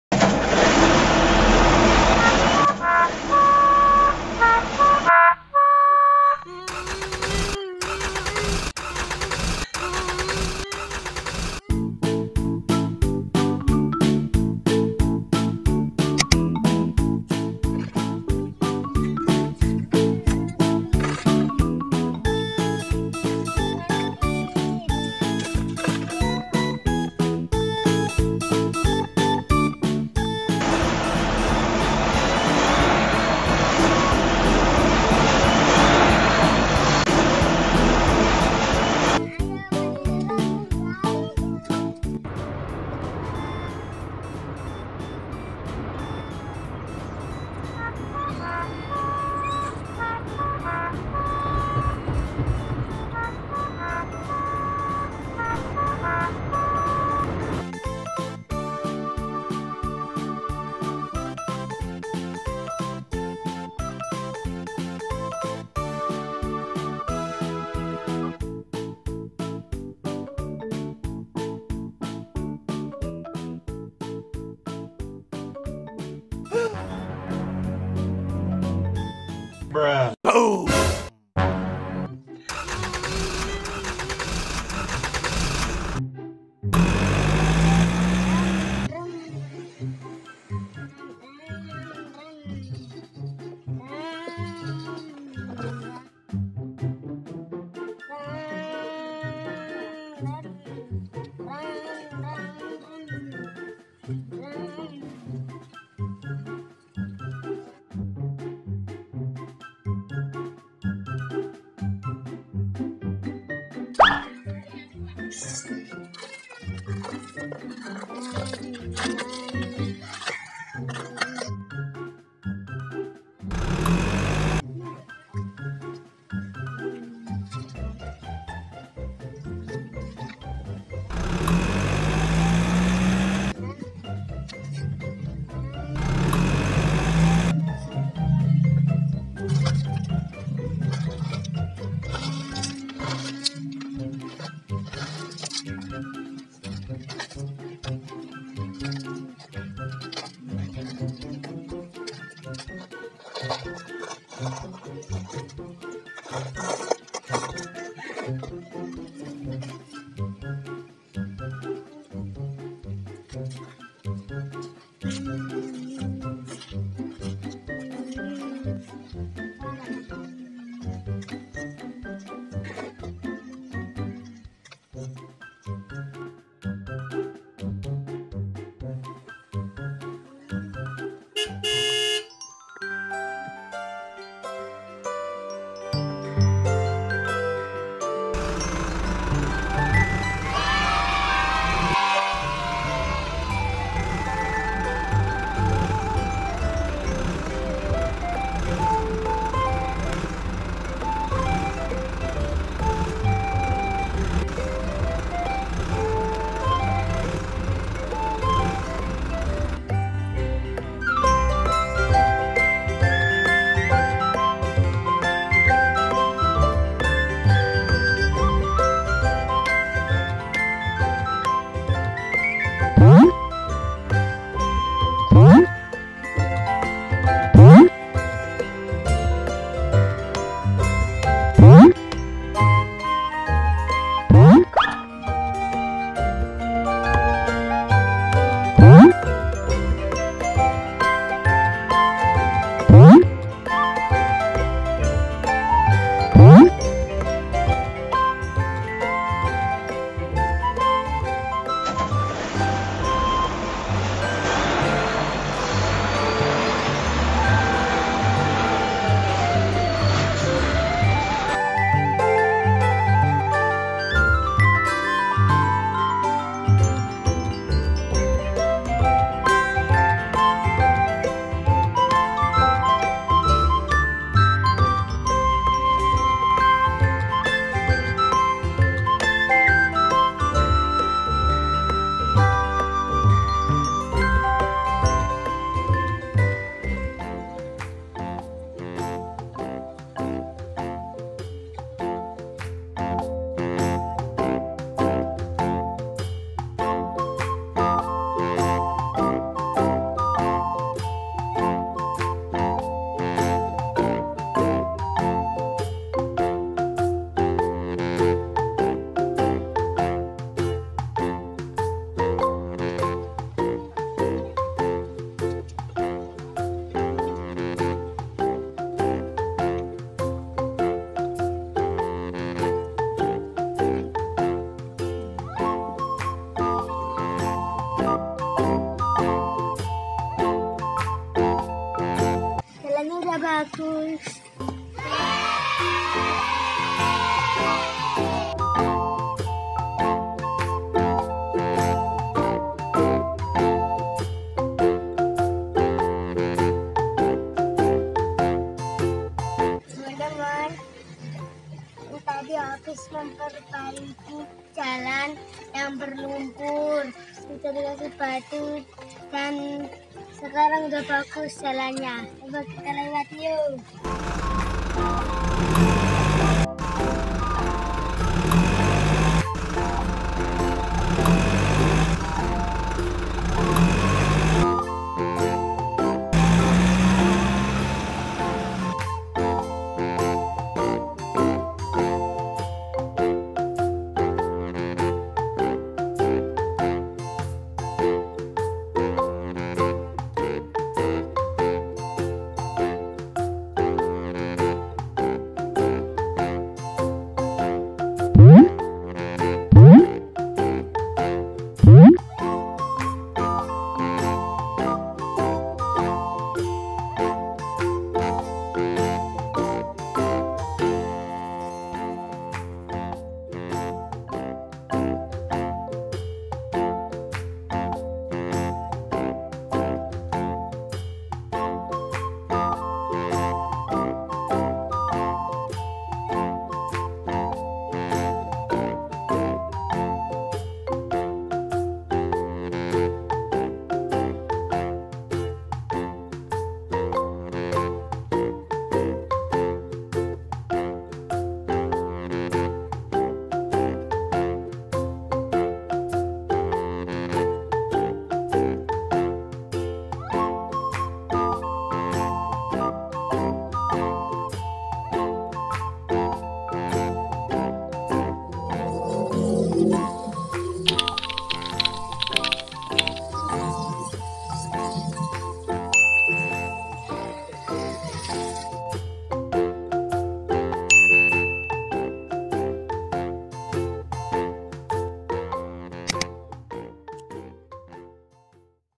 Mobil Truk Macet di Lumpur , Truk Plastik
Di video ini, Zafi bermain dengan mobil truk plastik yang macet di lumpur! Saksikan bagaimana truk mainan ini berjuang untuk keluar dari lumpur dengan aksi seru yang pasti menghibur.